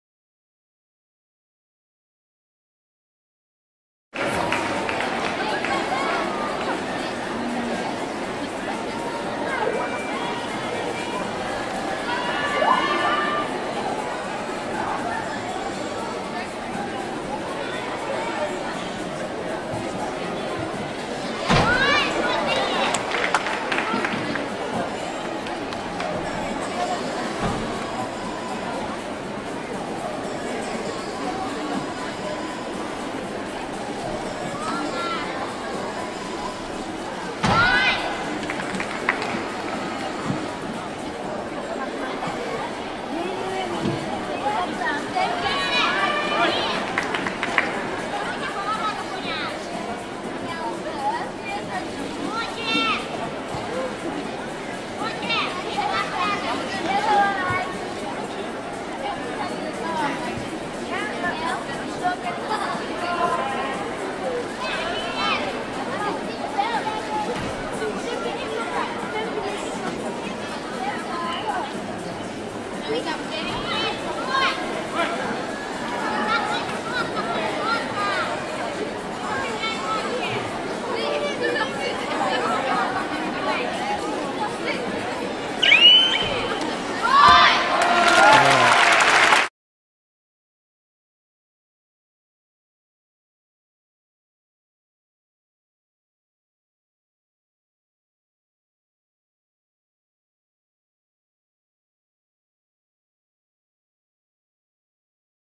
Ksenia Afanasyeva 2010 Beam D Score
I think that the turn connection and the split leap 1/1 were not counted.